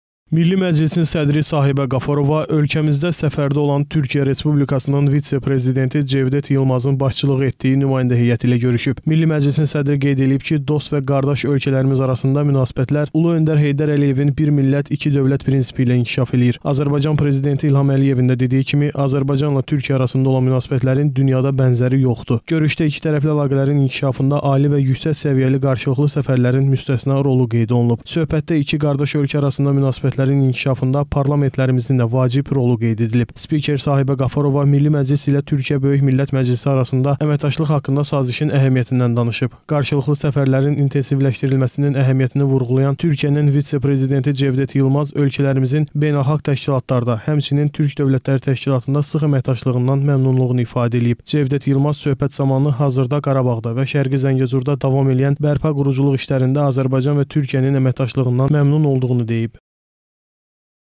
Sahibə Qafarova Cevdet Yılmazla görüşüb
İki qardaş ölkə arasında qarşılıqlı əməkdaşlıq müzakirə edilib